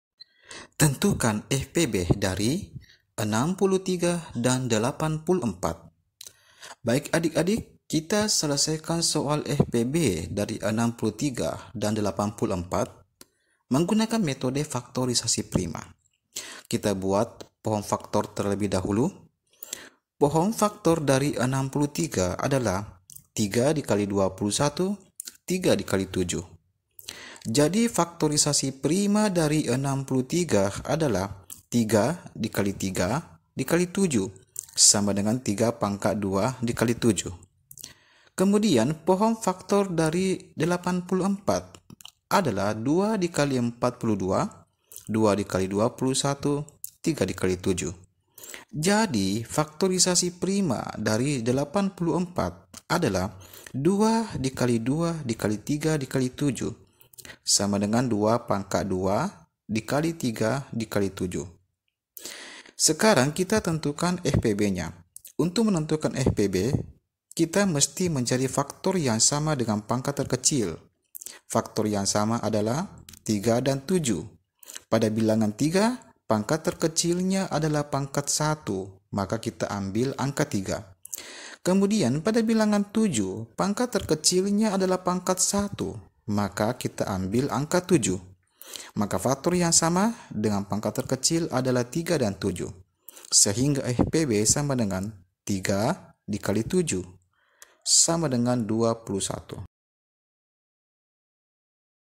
Tentukan FPB dari 63 dan 84 menggunakan pohon faktor - Menentukan FPB dari 63 dan 84
MATA PELAJARAN MATEMATIKA SD/MI
Menentukan FPB dari Dua Bilangan
Menentukan FPB dari 63 dan 84 meggunakan pohon faktor atau faktorisasi prima